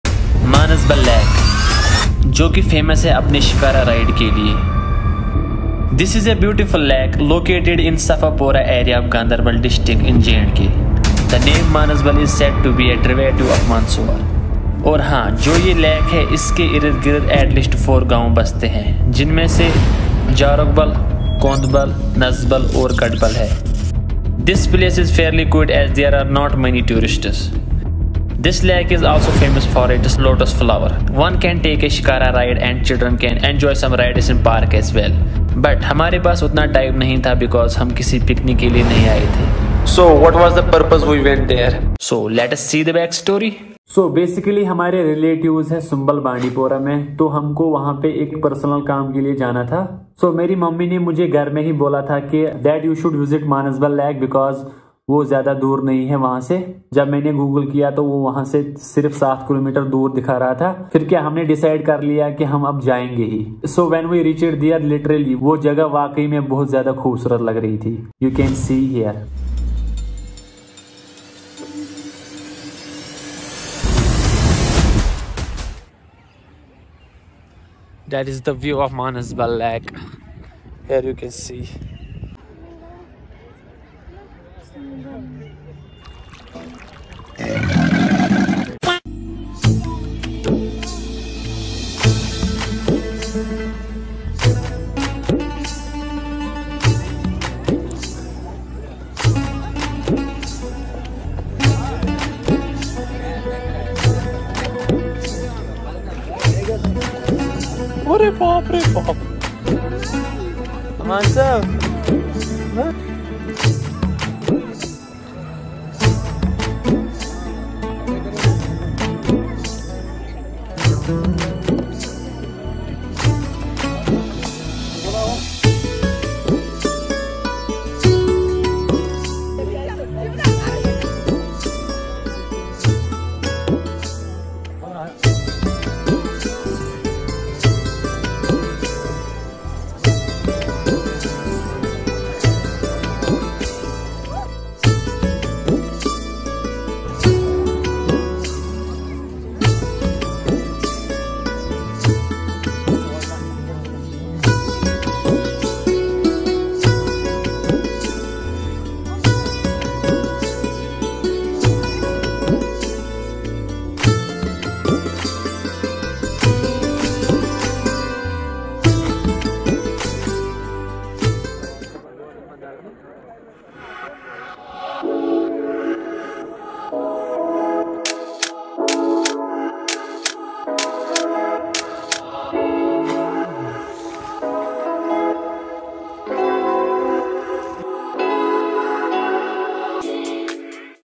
Manasbal Lake | The DEEPEST lake in India 🤯
Manasbal Lake is a freshwater lake located in Safapora area of Ganderbal District in Jammu and Kashmir, India. The name Manasbal is said to be a derivative of Manasarovar.
(All you need to know BEFORE you go)

One hour drive from Srinagar.
From Ganderbal you leave the highway and take village road. Road is narrow but good and one can enjoy the countryside.
Can combine with visit to Kheer Bhawani temple and Wular lake.
One can view the lake from the park or from outside the park.
There are a number of entries to the park. Entry fee is Rs 24 per person.
For better view and activities it is better to enter the park.
Park has enough shade and benches to sit.
The place is fairly quiet as there are not many tourists. Lake is famous for its Lotus flowers.
One can take a Shikara ride and children can enjoy some rides in the park on payment basis.
There is a restaurant inside the park and a food court outside the park. There were very few options for eating as most food stalls were closed.
It is advised that one should carry your own food as good food may not be available on this circuit.
There is an ancient submerged Shiva temple on one end of the lake which can be approached from outside the park also. A caretaker is available who for a tip will take you inside the locked premises to feed fishes.
Time required around one hour, more if you take a Shikara ride (Rs 400 for one hour)

Drone shots by - @imadclicks
Script writer-@Kashurtalkshow
Voice -@Kashurtalkshow
Editor-@Kashurtalkshow
Camera- iPhone 11
Mic- smartphone